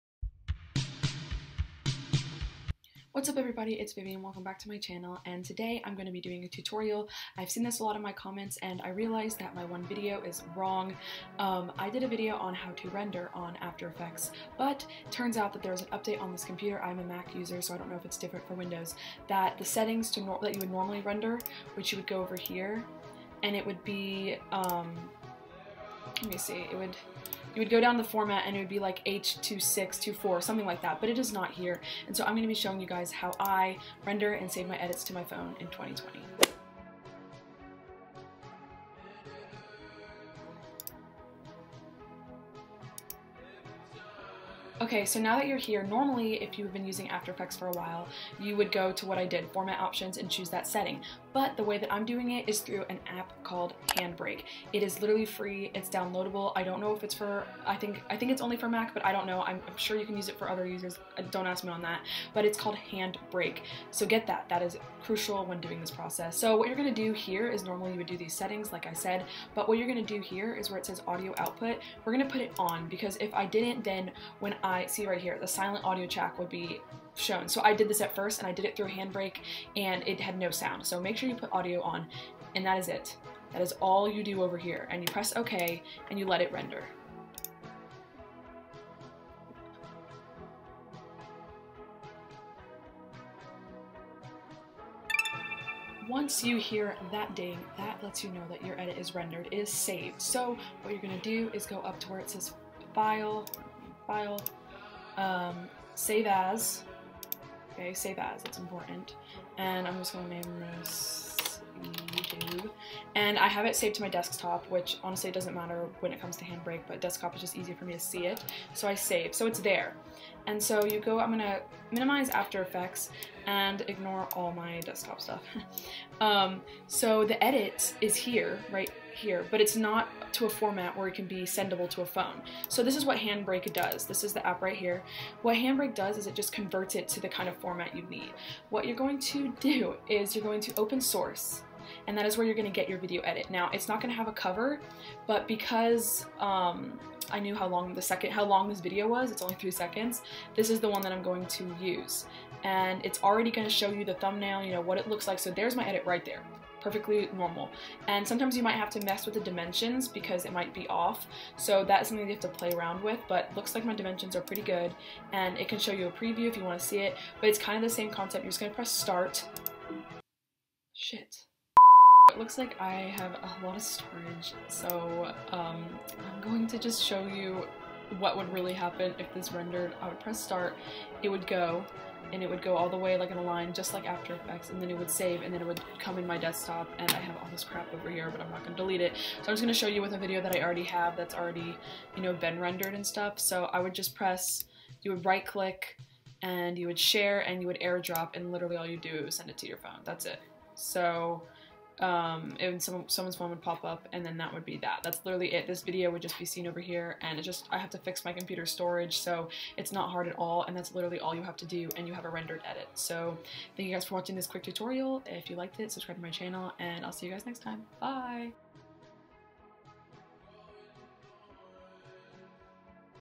how to render + transfer after effects edits 2020| Handbrake tutorial
HOW TO RENDER ON ADOBE AFTER EFFECTS 2020 MAC CATALINA UPDATE!
So I know I have already uploaded this video, but a lot of you have been commenting that there isn't the H.264 option. Here is the updated rendering and importing to your iPhone tutorial!

MORE TAGS: after effects, after effects edit, tutorial, rendering tutorial, after effects tutorial for beginners, video star, charli damelio, tik tok, instagram